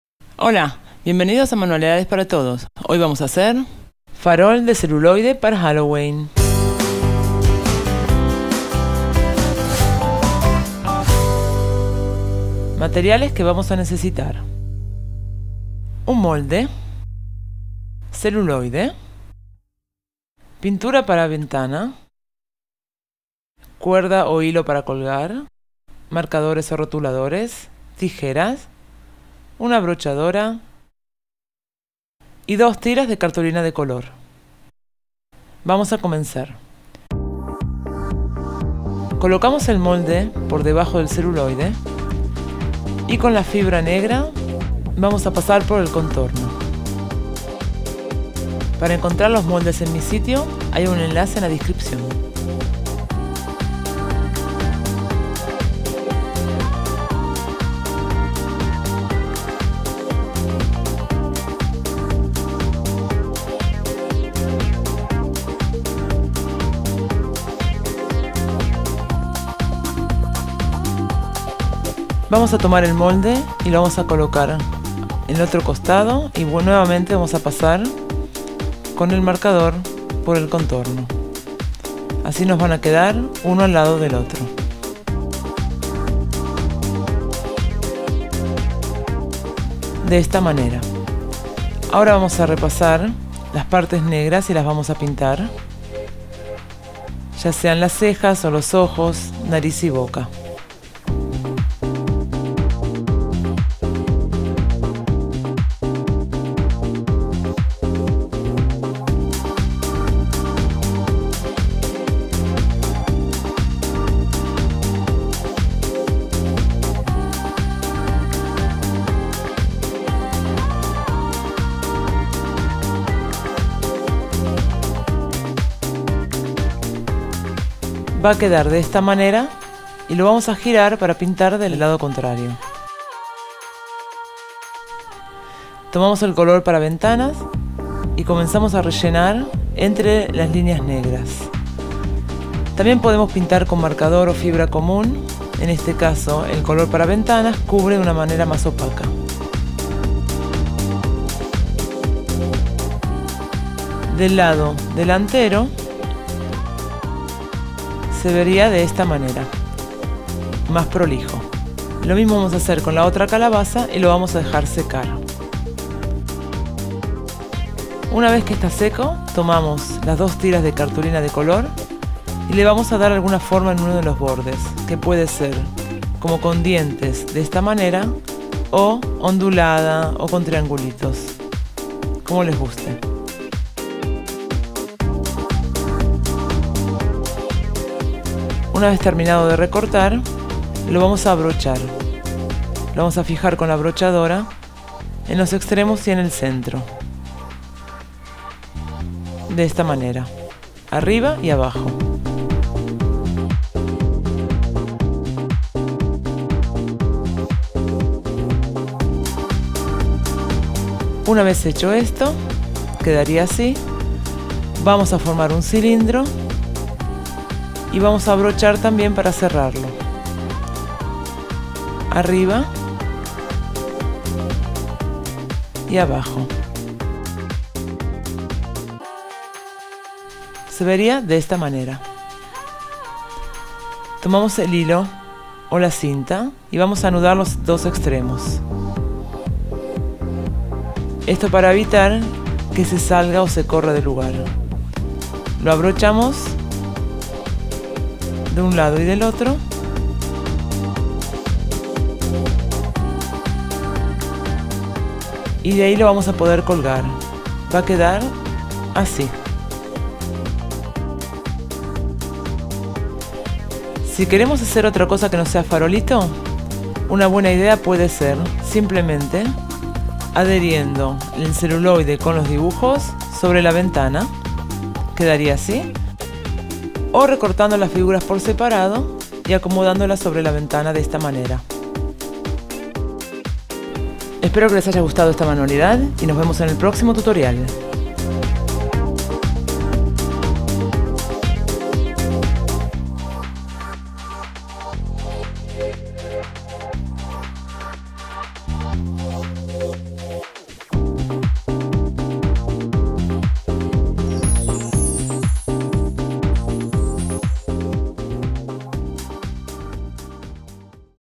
Manualidades para Halloween - Farolito de celuloide - Manualidades para todos
En este tutorial te enseño a hacer un farolito de celuloide para colgar o pegar al vidrio de la ventana, decorativo típico de Halloween. Un adorno especial para el día de los muertos, fácil de hacer.